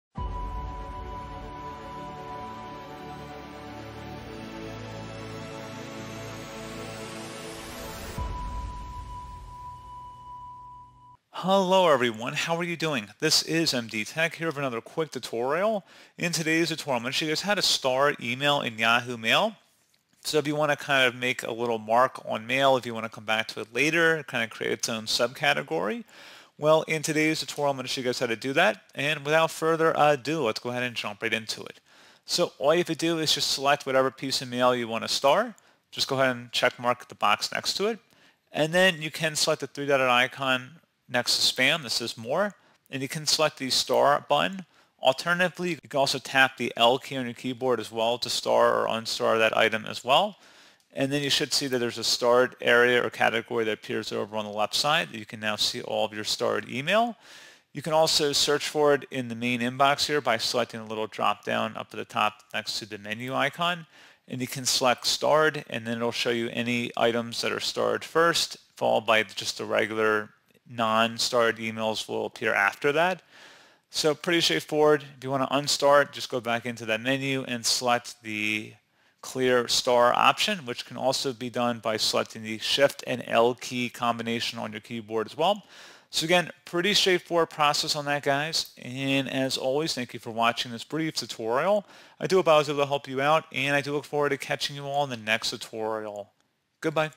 How To Star Email In Yahoo Mail [Guide]
This tutorial describes how to use Yahoo Mail's star system to see only your important Yahoo Mail emails and not those that can wait until later. You may also sort messages by certain criteria to find important messages without sifting through hundreds of others.

To star an email in Yahoo Mail, you can click the star icon to the left of the subject line in your inbox. Starring emails is a way to keep important messages in one place and easily access them later.

Issues addressed in this tutorial:
how to star email in yahoo
how to start an email in yahoo
what does it mean to star an email in yahoo